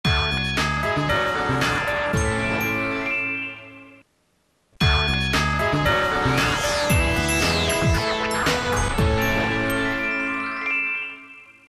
Bumpers de LS 83 TV Canal 9 Buenos Aires - Navidad 2010
Separadores del espacio publicitario de Canal 9 con la gráfica navideña.